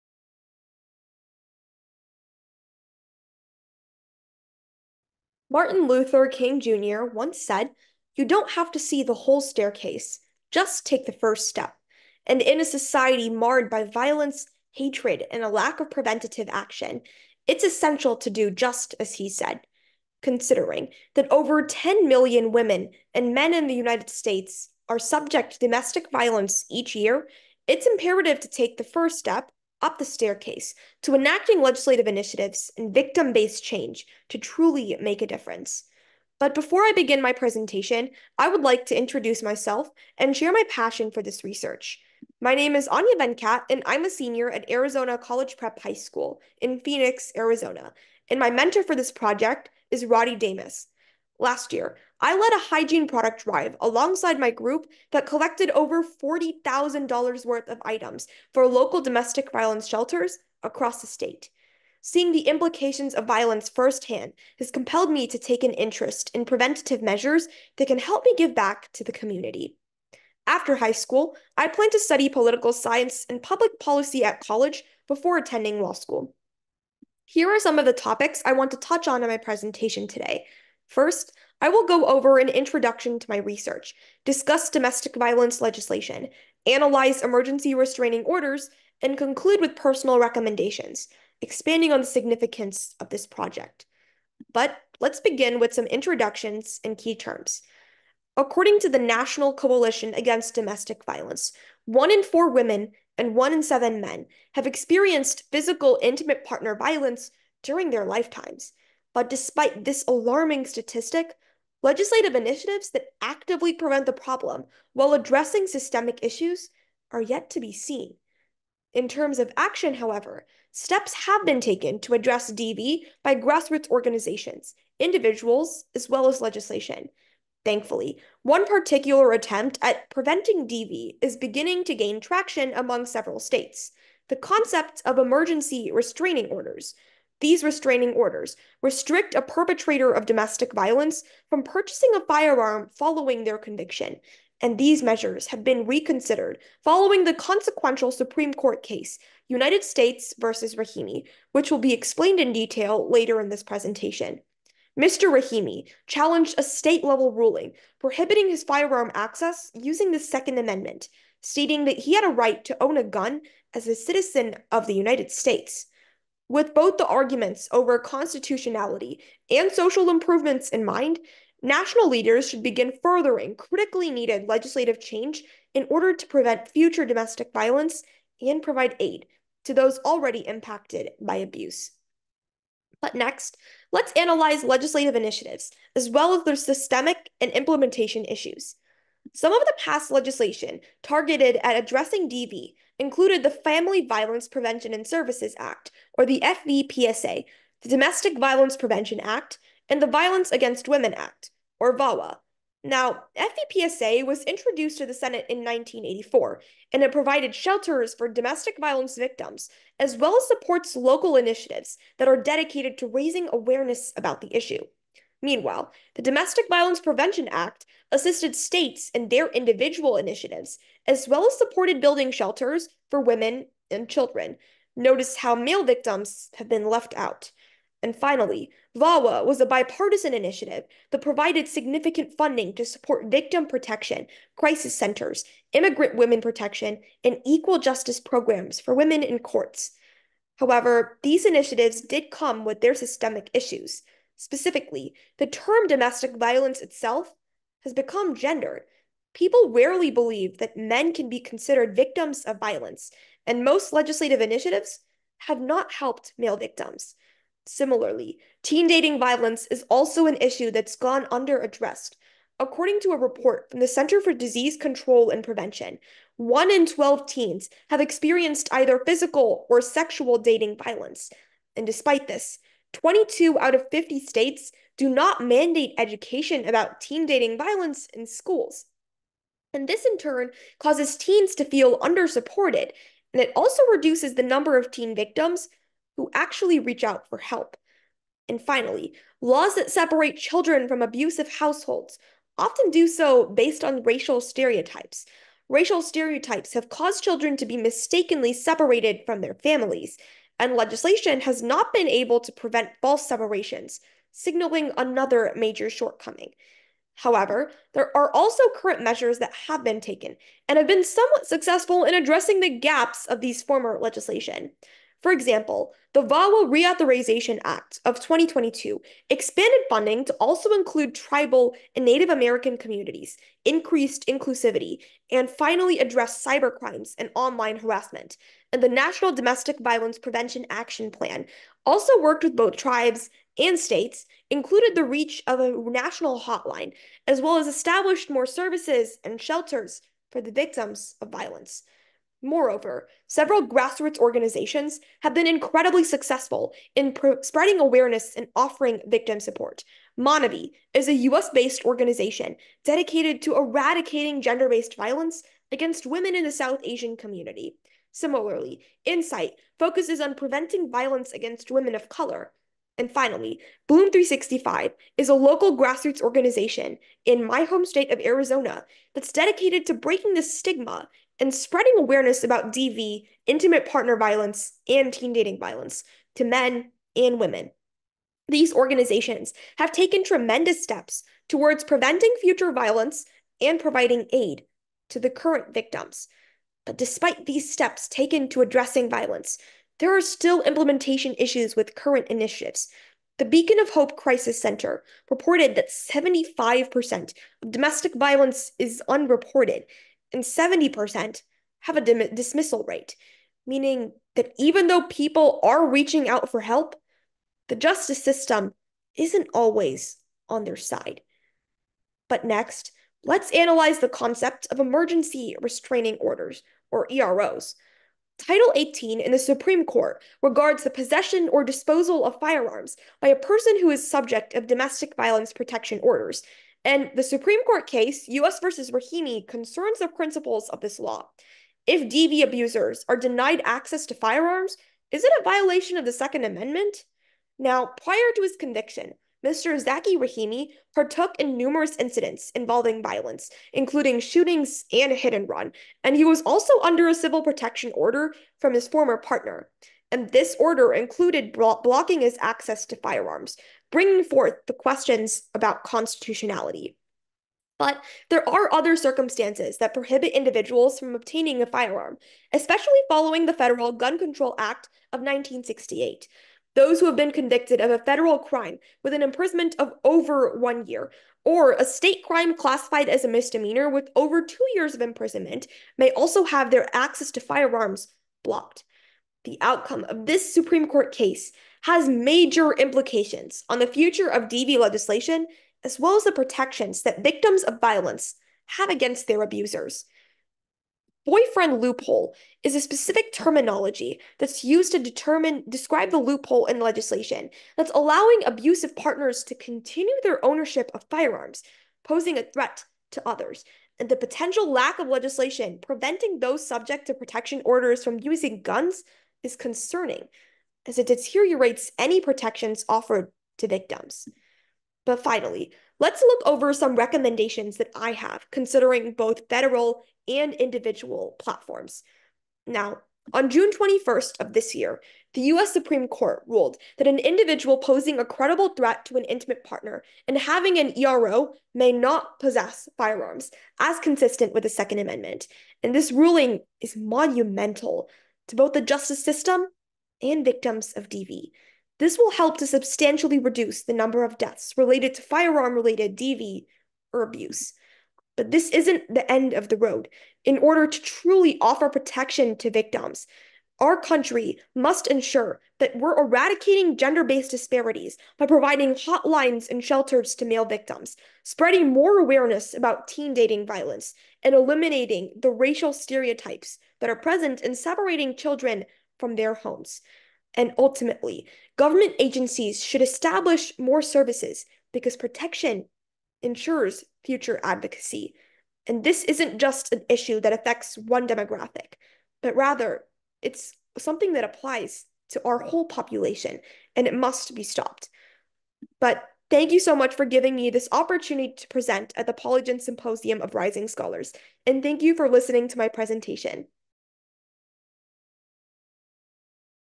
Domestic Violence Laws and Restraining Orders Presentation by Aanya Venkat at Polygence's Symposium
‘Preventative Domestic Violence Legislation Through the Years: Is Change Needed?‘ was a Humanities Conference Talk delivered asynchronously by Aanya Venkat at Polygence’s 11th Symposium of Rising Scholars in September 2024.